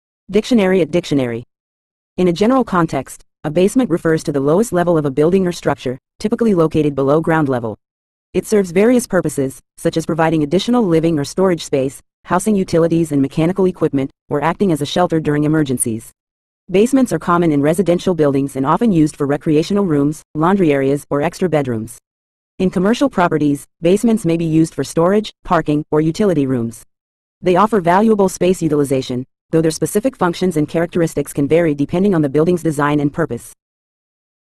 Basement Meaning In English
In a general context, a basement refers to the lowest level of a building or structure, typically located below ground level. It serves various purposes, such as providing additional living or storage space, housing utilities and mechanical equipment, or acting as a shelter during emergencies. Basements are common in residential buildings and often used for recreational rooms, laundry areas, or extra bedrooms. In commercial properties, basements may be used for storage, parking, or utility rooms. They offer valuable space utilization, though their specific functions and characteristics can vary depending on the building's design and purpose.